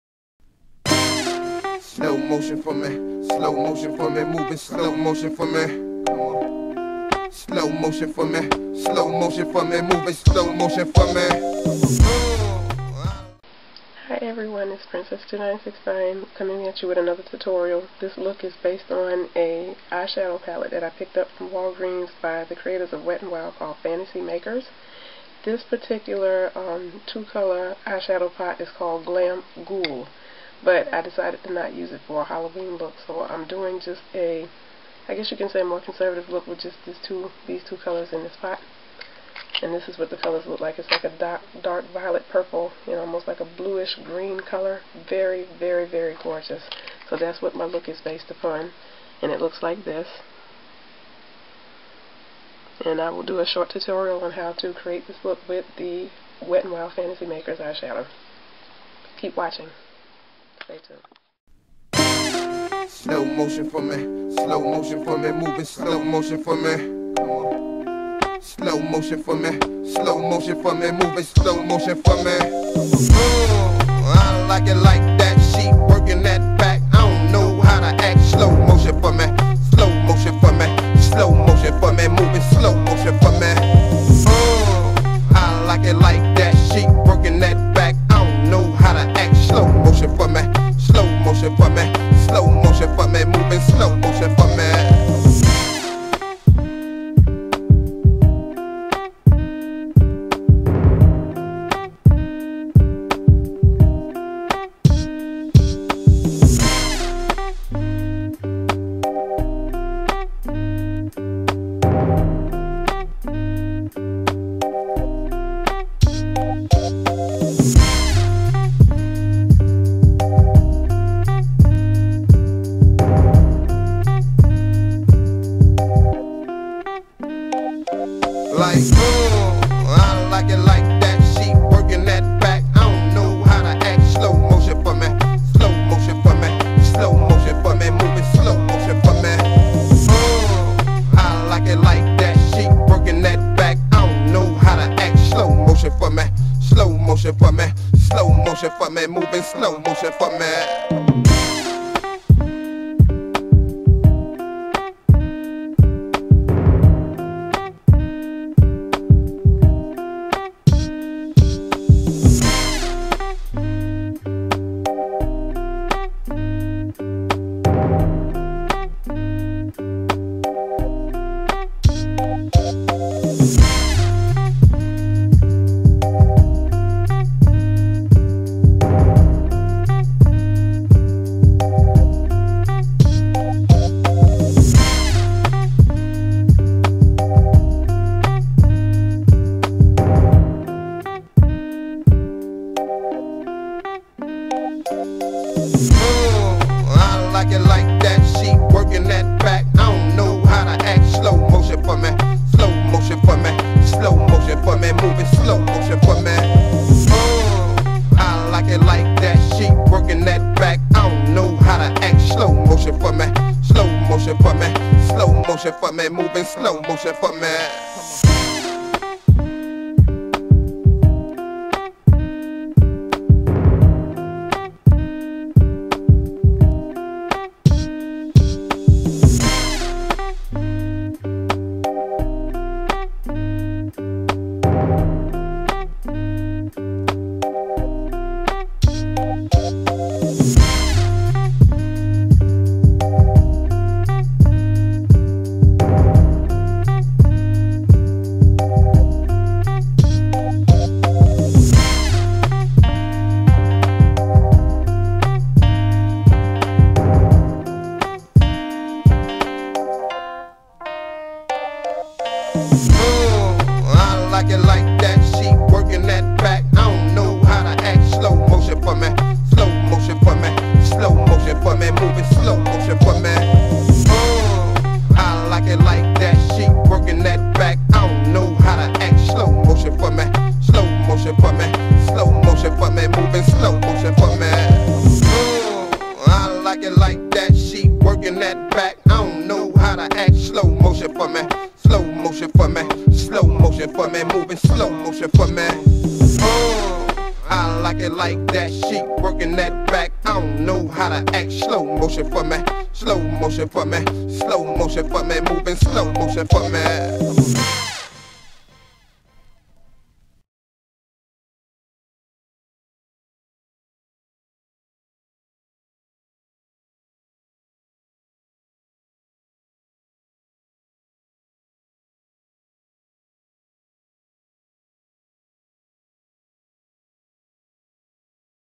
Wet N' Wild Fantasy Eyes Look
A Look I created using The Wet 'N Wild Fantasy Eyeshadow Pot (for Halloween) and Coastal Scents 88 Matte Palette!
Other Products Used:
1. Revlon Decrease Eyeshadow Primer Base
2. DermaBlend Cover Creme Foundation in Golden Bronze
3. Revlon Mineral Finishing Powder in Sunkiss
4. NYX Lipstick in Luster
5. Jordana Lip Pencil in Sweet Plum
6. Iridescent Orange Blush (10 Color Blush Palette)
7. Milani Pressed Powder in Ginger (cheek contour)